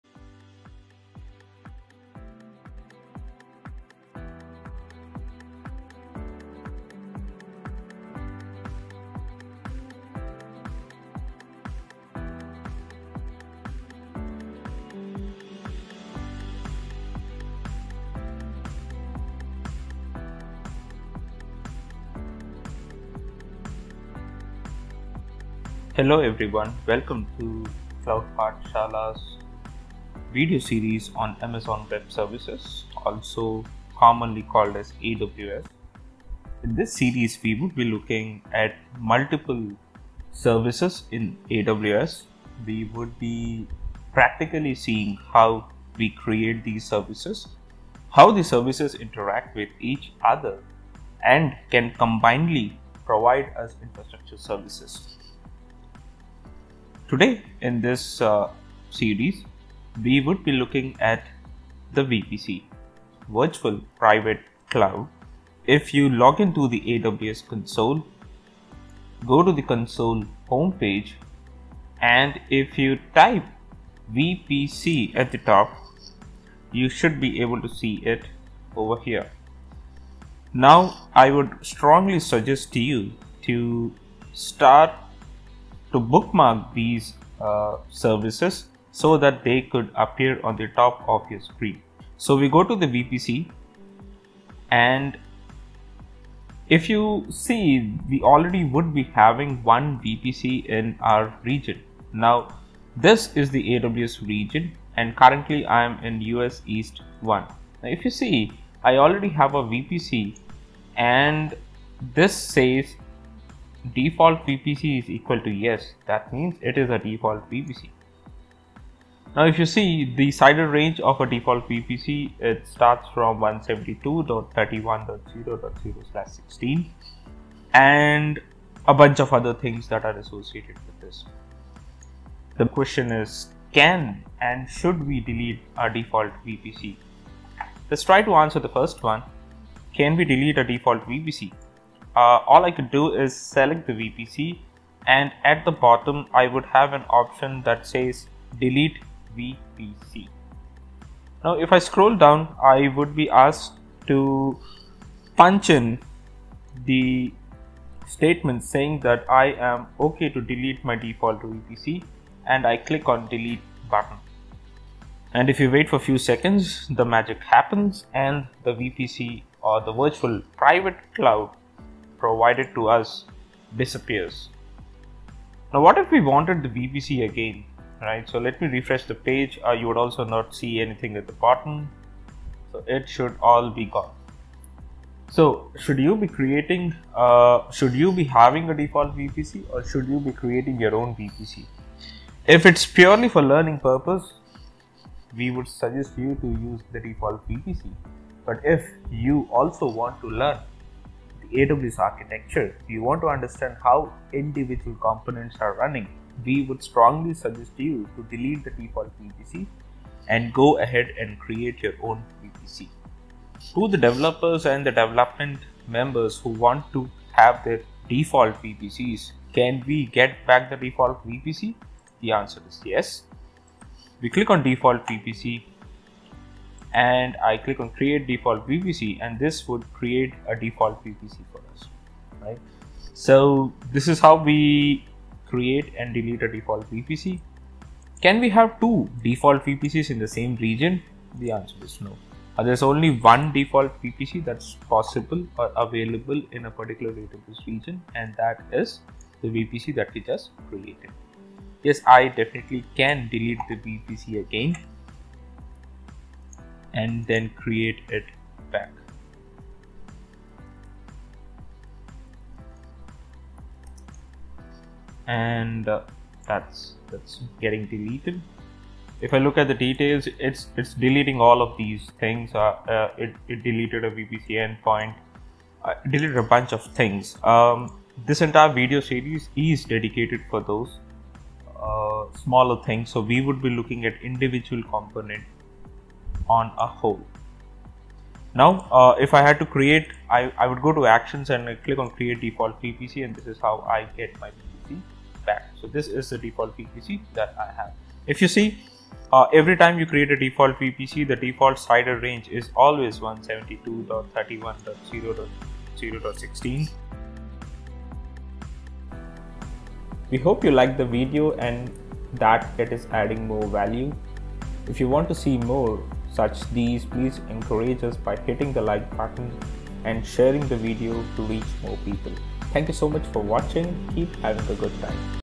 Why you should delete the "Default VPC" in your region. | How to Delete default VPC.
cp-video-series-aws 01

We discuss on Deletion and Creation of Default VPC in AWS Regions.
The bigger questions is, should we continue to use AWS Default VPC or do we create our custom VPC.

We answer the tough questions on Should We ? Can We Delete ? Can we Create/Re-Create Default VPC.

We also undertake Courses in AWS Cloud and DevOps tools.